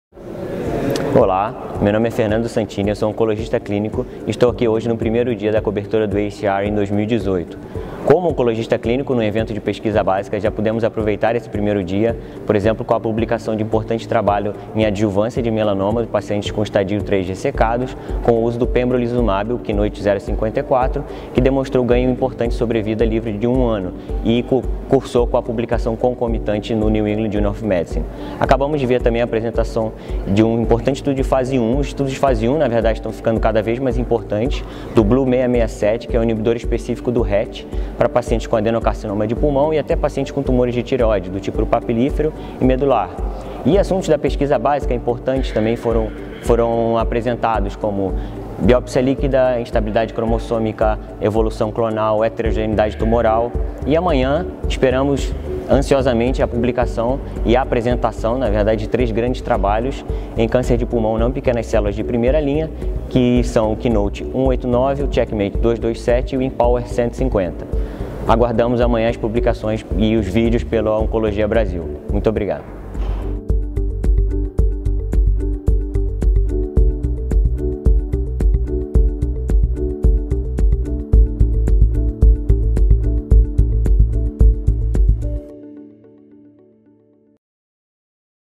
AACR 2018 - Dr. Fernando Santini
Dr. Fernando Santini cita alguns destaques da convenção anual da Associação Americana para Pesquisa do Câncer.